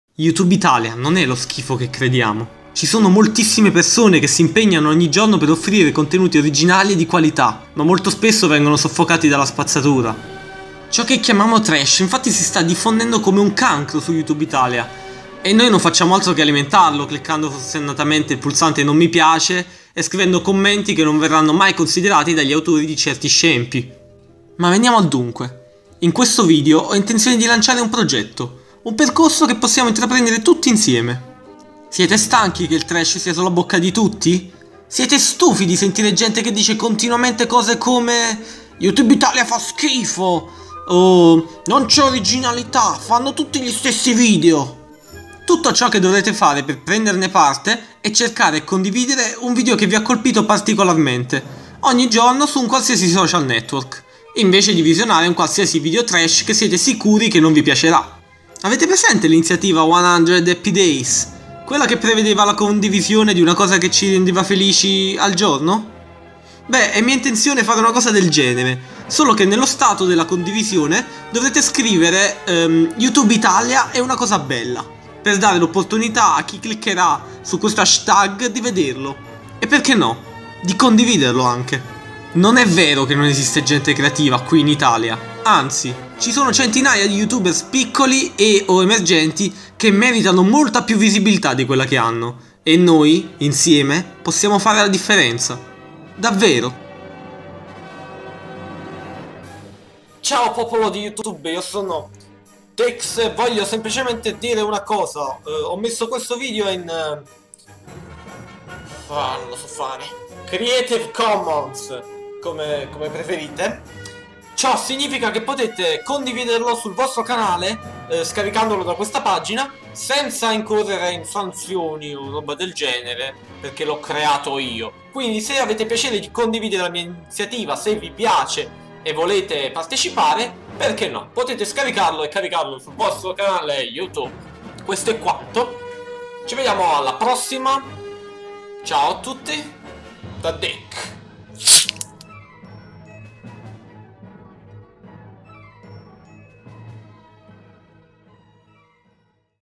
EXTRA - YoutubeItaliaèunacosabella
Youtube Italia non è lo schifo che crediamo. Ci sono moltissime persone che s'impegnano ogni giorno per offrire contenuti originali e di qualità, ma molto spesso vengono soffocati dalla spazzatura.
Ciò che chiamiamo trash infatti si sta diffondendo come un cancro su Youtube Italia, e noi non facciamo altro che alimentarlo, cliccando forsennatamente il pulsante "non mi piace" e scrivendo commenti che non verranno mai considerati dagli autori di certi scempi.
Ma veniamo al dunque: in questo video ho intenzione di lanciare un progetto, un percorso che possiamo intraprendere tutti insieme. Siete stanchi che il trash sia sulla bocca di tutti? Siete stufi di sentire gente che dice continuamente cose come 'Youtube Italia fa schifo' o 'Non c'è originalità: fanno tutti gli stessi video'? Tutto ciò che dovrete fare per prenderne parte è cercare e condividere un video che vi ha colpito particolarmente ogni giorno su un qualsiasi Social Network, invece di visionare un qualsiasi video trash che siete sicuri che non vi piacerà. Avete presente l'iniziativa '100 Happy Days'? Una cosa del genere, solo che nello stato della condivisione dovrete scrivere YoutubeItaliaèunacosabella, per dare l'opportunità a chi cliccherà su questo hashtag di vederlo e, perché no, di condividerlo anche.
Non è vero che non esiste gente creativa qui in Italia, anzi: ci sono centinaia di Youtubers piccoli e/o emergenti che meritano molta più visibilità di quella che hanno, e noi, insieme, possiamo fare la differenza, davvero.